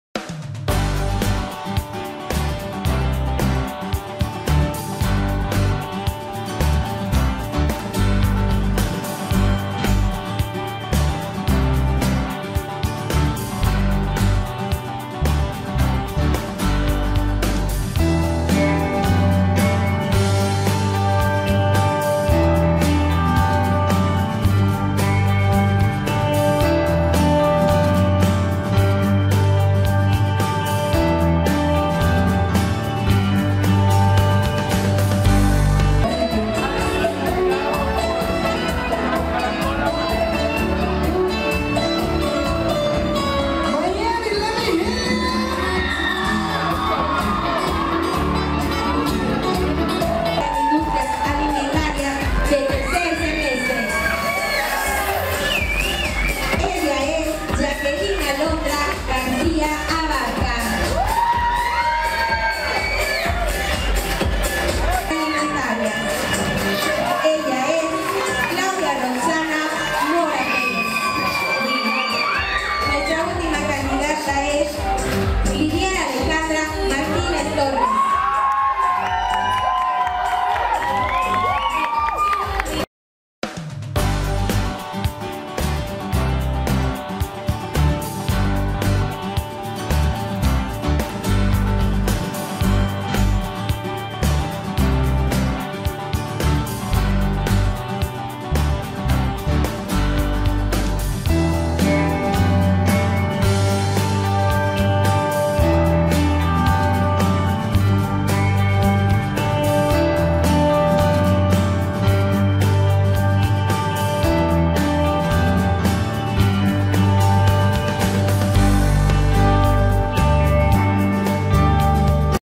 Arandas -Presentan Candidatas ITSA.
Presentan candidatas a Señorita ITSA en pasado Viernes en Palazzo.

Te invitamos al Certamen el 10 de Octubre en La Terraza Hacienda Vieja.